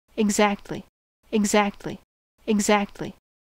How to Say or Pronounce Exactly
This video shows you how to say or pronounce Exactly.

How accurate does it say Exactly? How would you say Exactly?